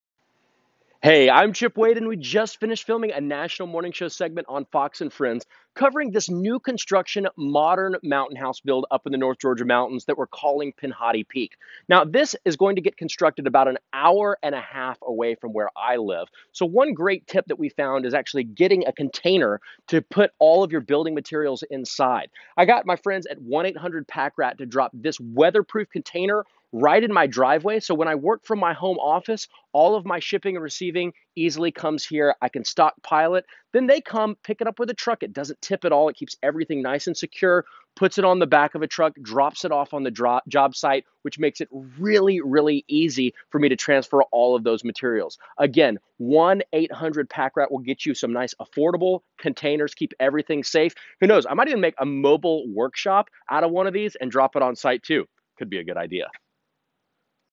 Chip Wade Features 1-800-PACK-RAT on Fox & Friends: Behind The Scenes Footage
Chip Wade, owner of design and construction firm WadeBuilt LLC, gives you a behind the scenes look after his Fox & Friends segment about how he uses 1-800-PACK-RAT's portable storage containers for his work. Check out how he uses 1-800-PACK-RAT and see how we can make moving and storage simple for you to!